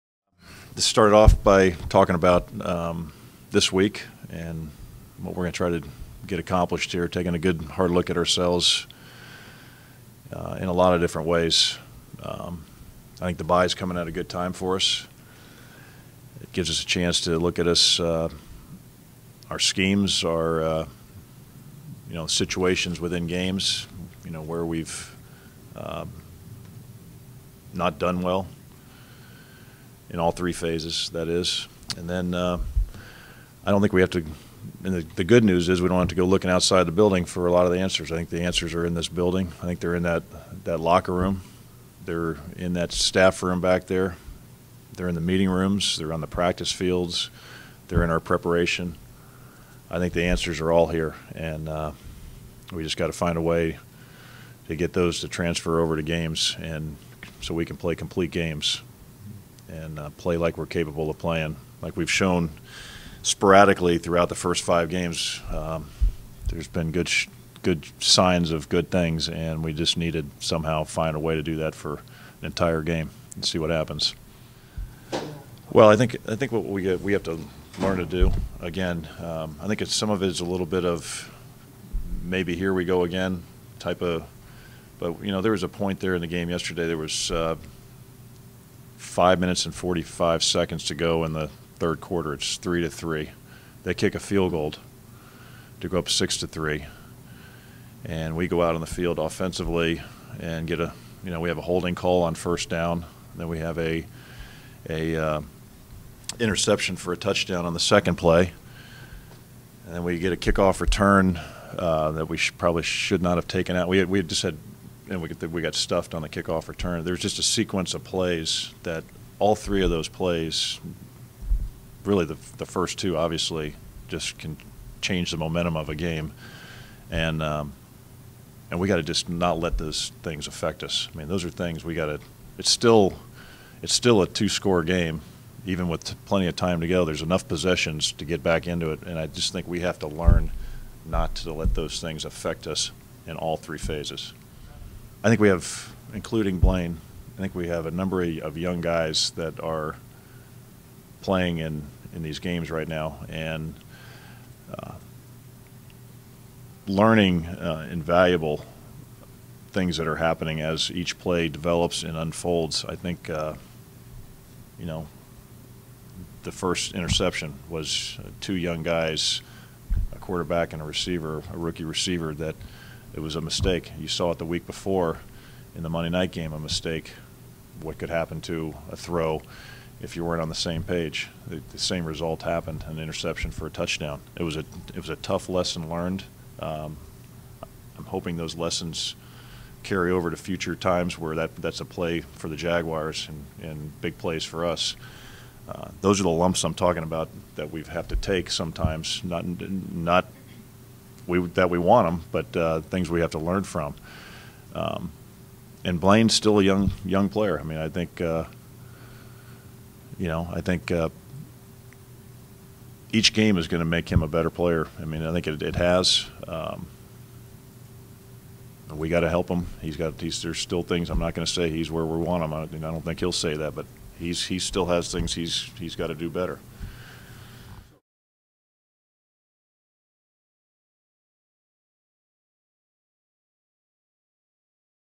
Mularkey Bears Press Conference
Jaguars head coach Mike Mularkey talks about Sunday's loss to the Chicago Bears.